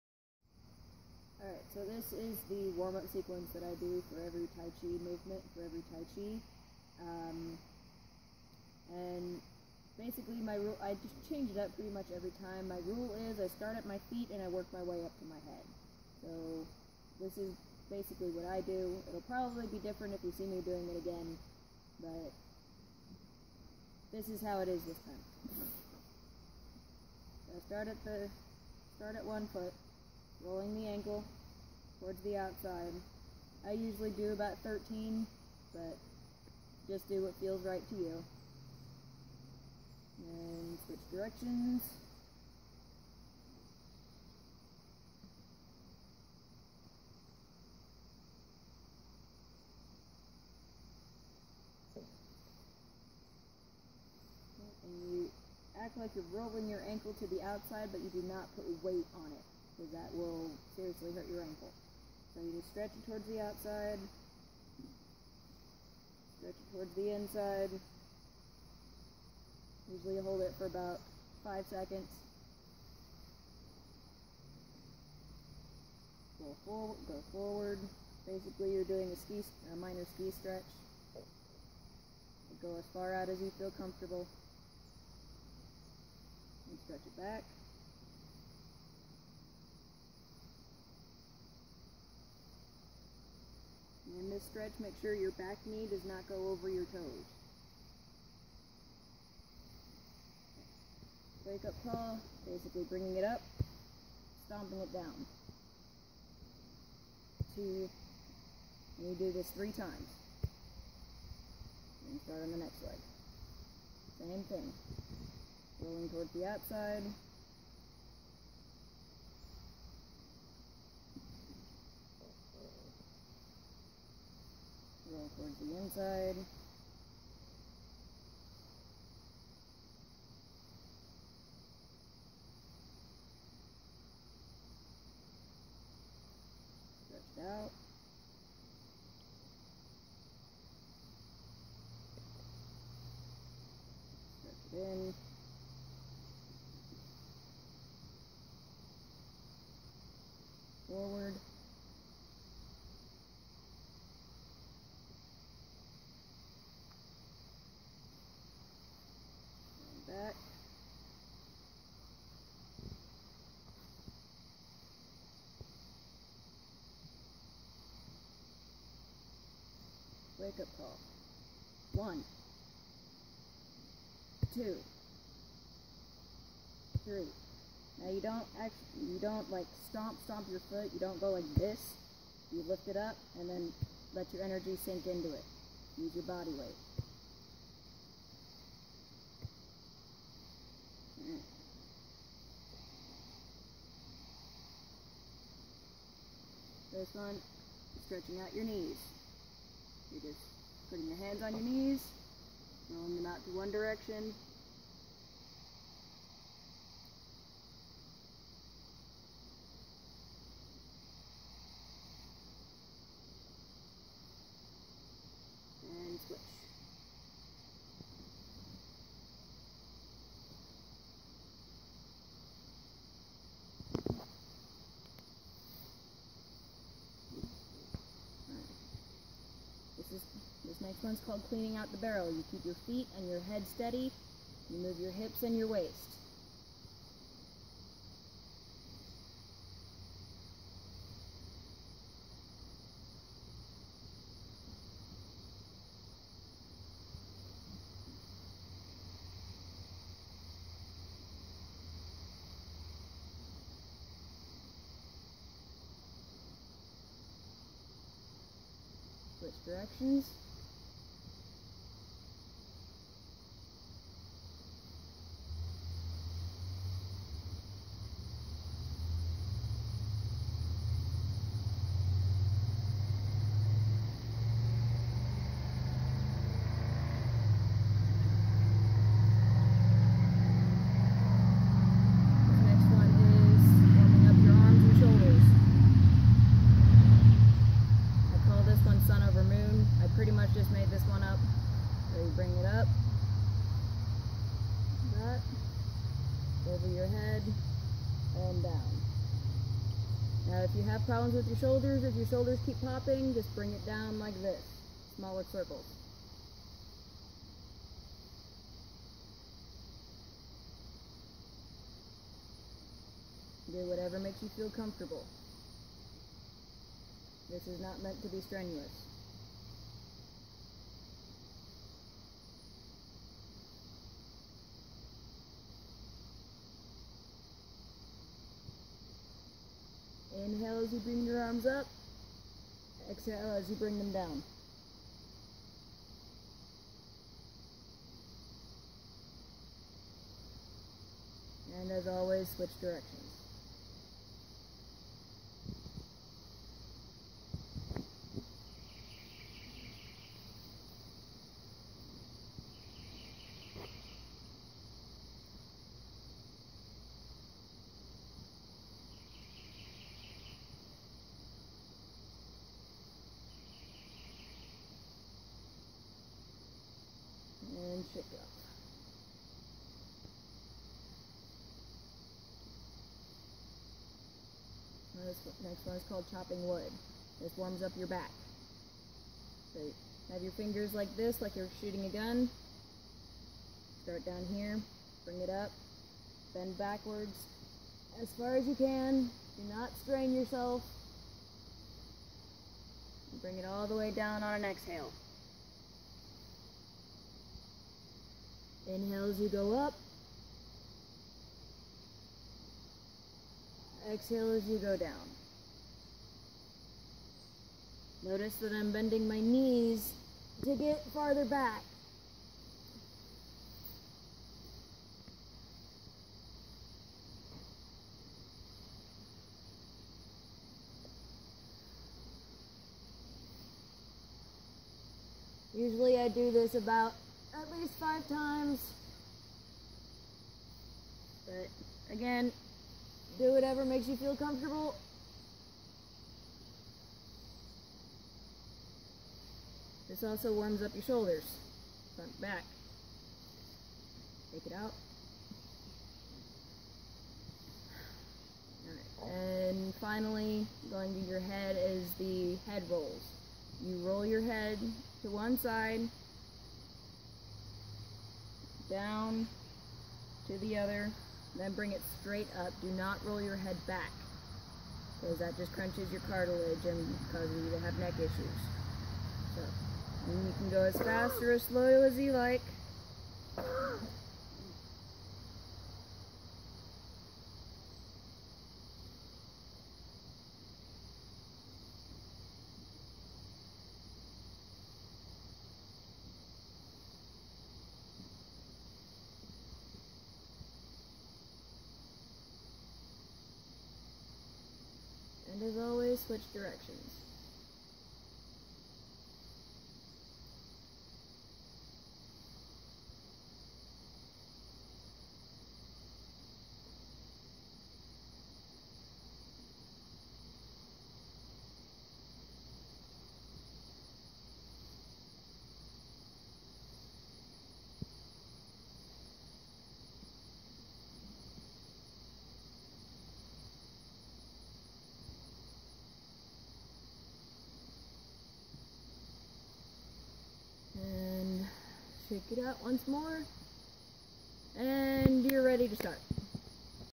Tai Chi Moving 5 Elements Warmup Sequence
Thanks to Rowan FaeFire for doing this video. The 5 elements of Tai Chi are a great way to balance, and prepare oneself for a busy day!